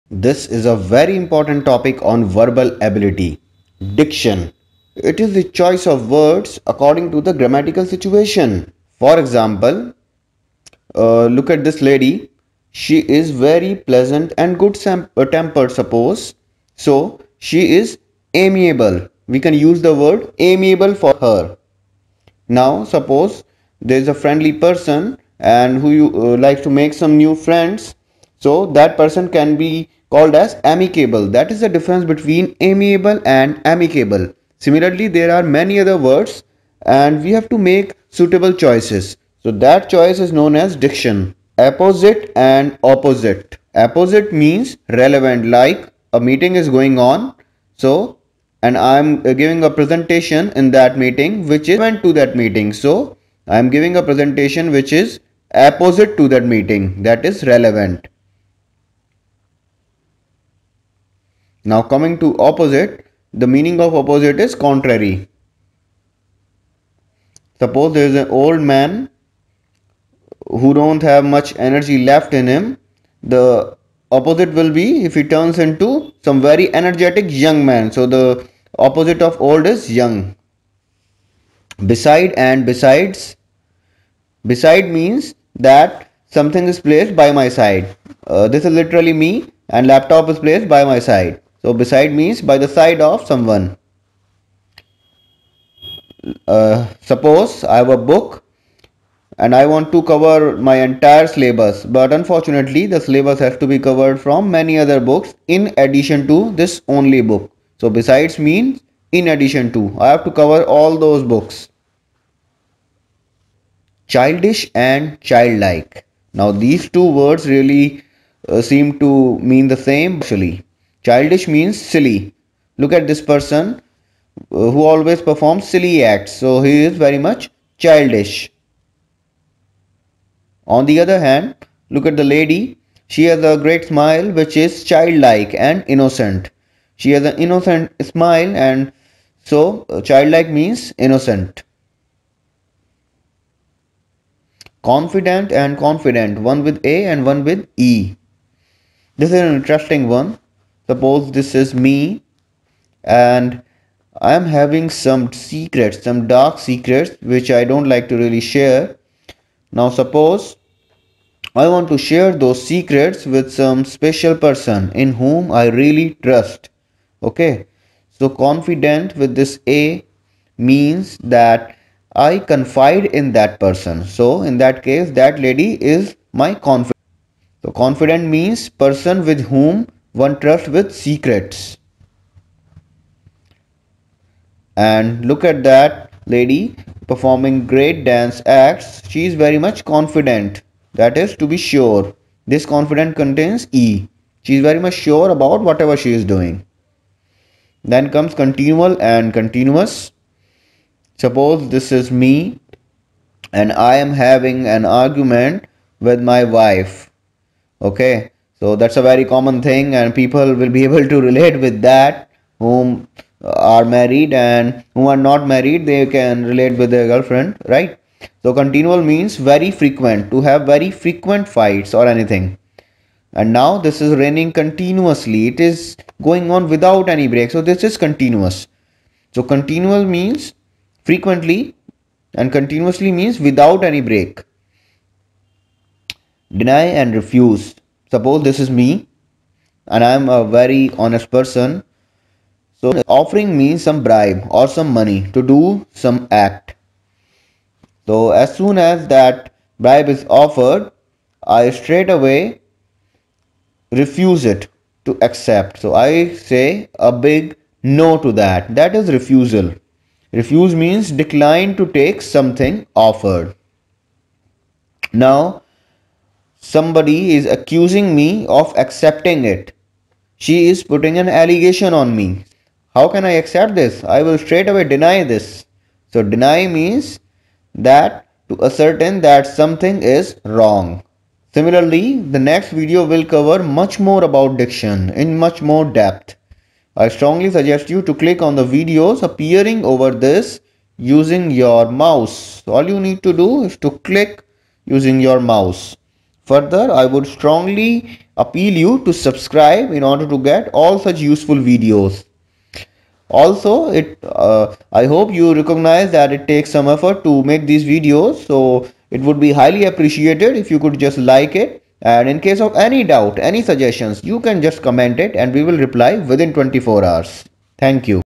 Diction : Video 2 | How to use correct words
This video is about correct use of words as per grammar.